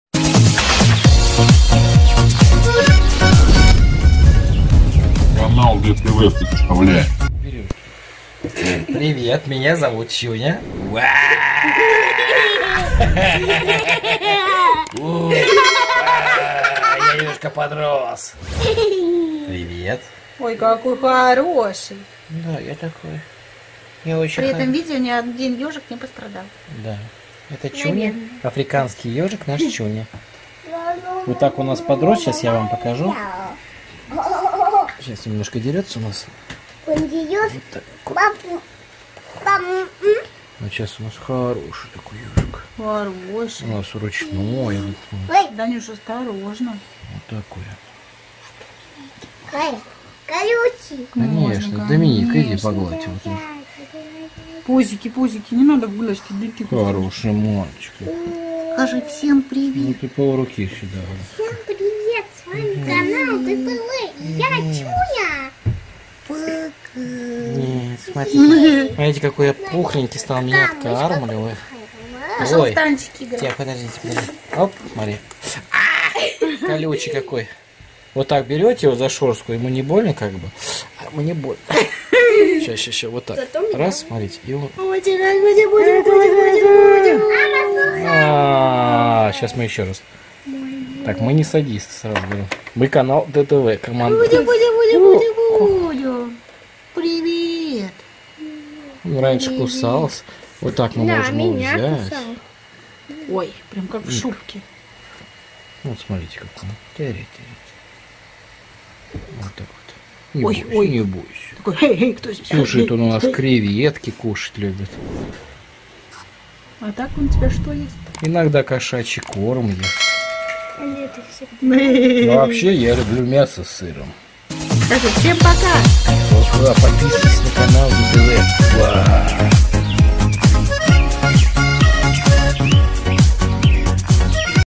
НАШ ДИКИЙ АФРИКАНСКИЙ ЕЖИК / ЧУНЯ / 4К / 2020
Если ты чужой, то он откусит твой палец, к Вашему вниманию Ежик "Чуня" ласковый мальчик для своих любимчиков )